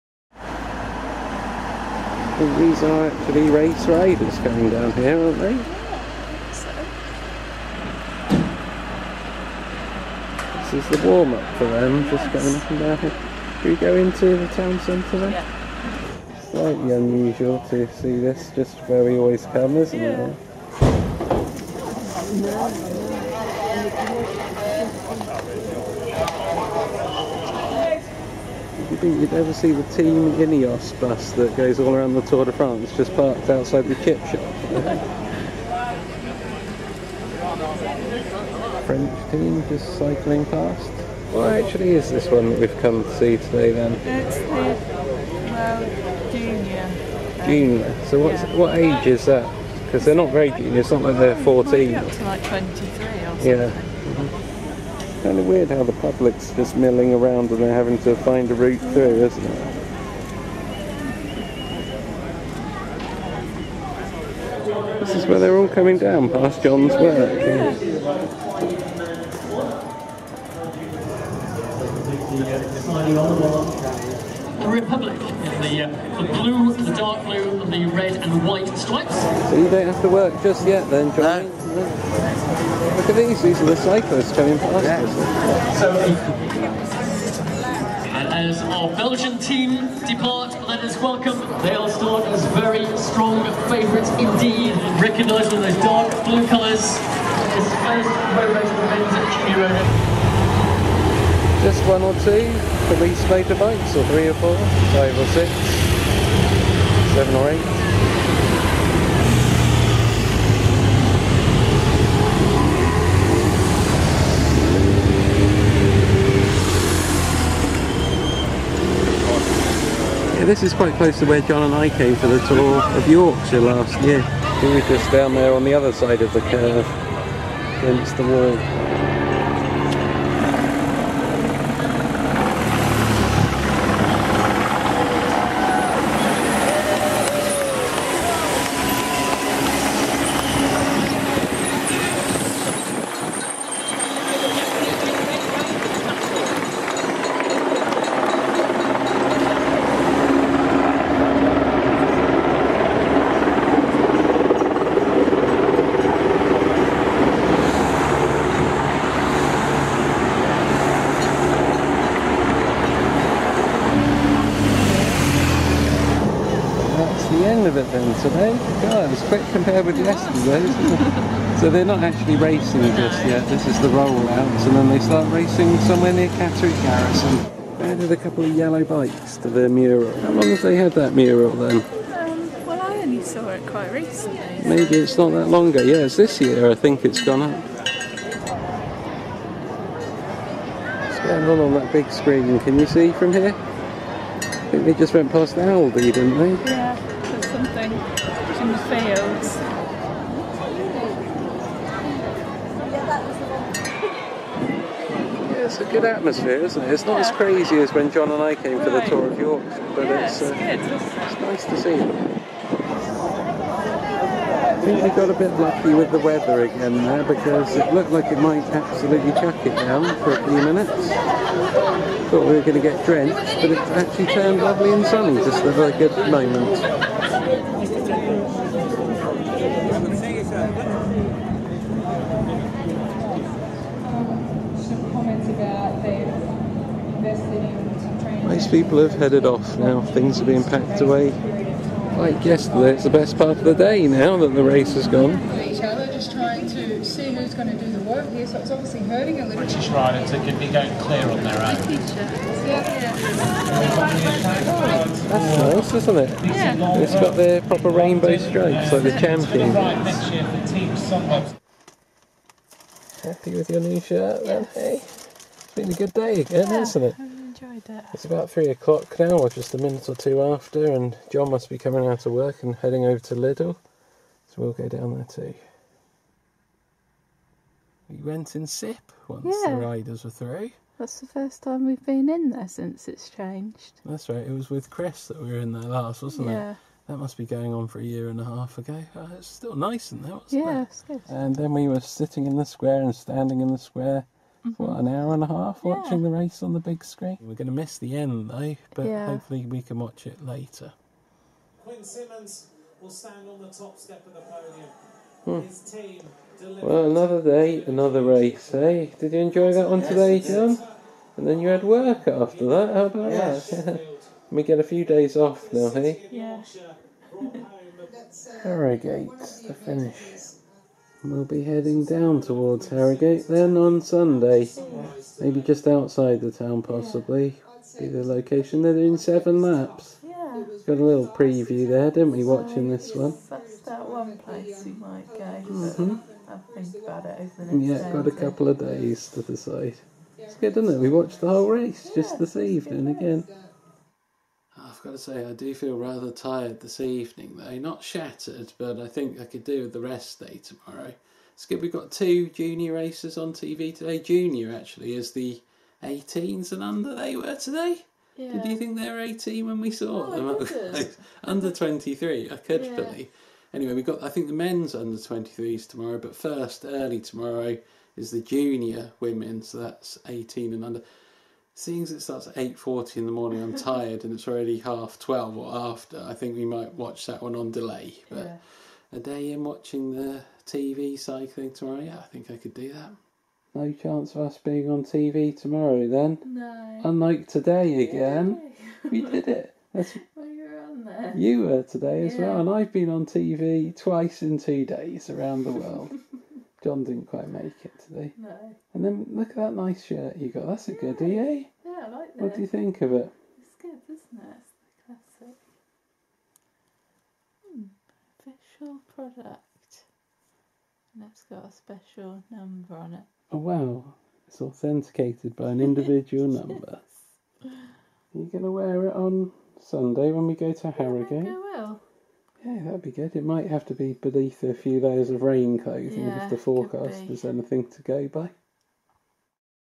26th September 2019
James & Christina are in Richmond to view the start of the junior men's road race at the UCI Road World Championships. John comes out of work for a few minutes to watch as well. After a look around town as the festivities are wrapped up, they catch up with a replay of the race at home.